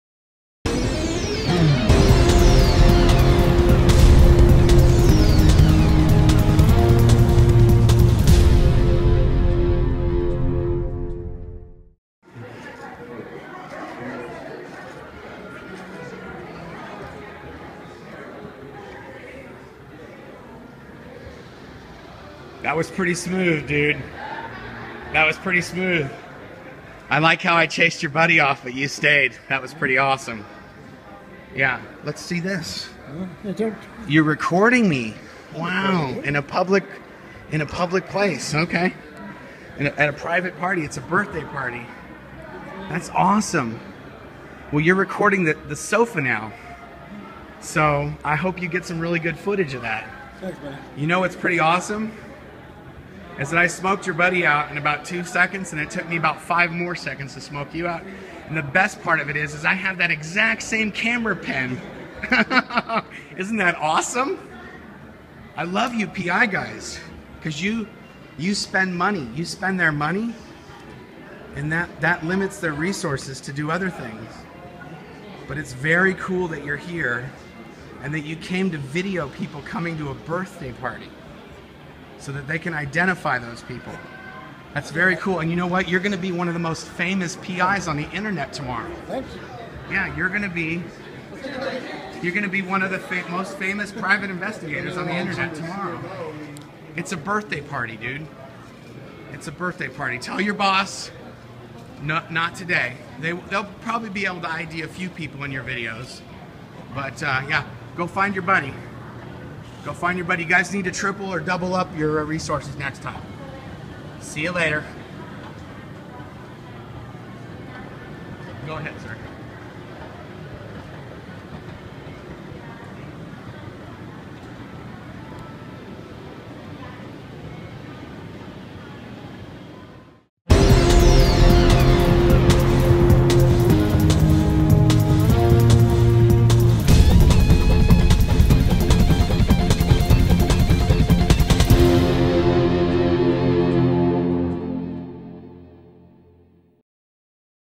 Scientology PI Gets Shut Down!
This is a video that I shot of a Scientology Private Investigator at a birthday party in Las Vegas.

All Paperback & hardback books sold from Blownforgood.com will be signed by Claire & Marc Headley.

Blown for Good Book - Audible